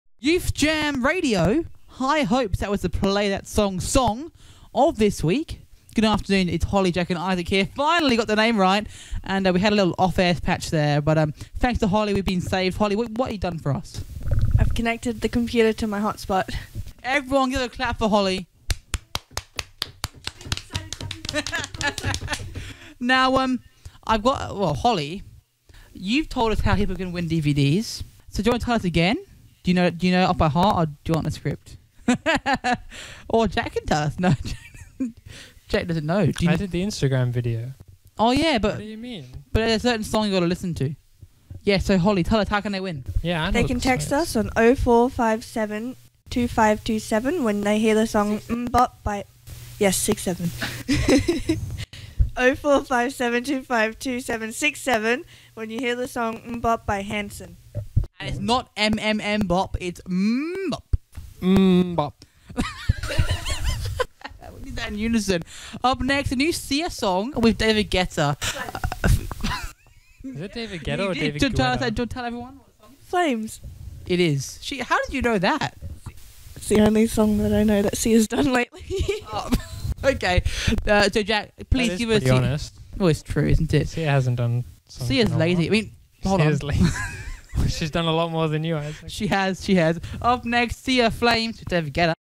Holly, Jack & Isaac: Mixed Podcast
This podcast features a weird conversation from 23rd June 2018.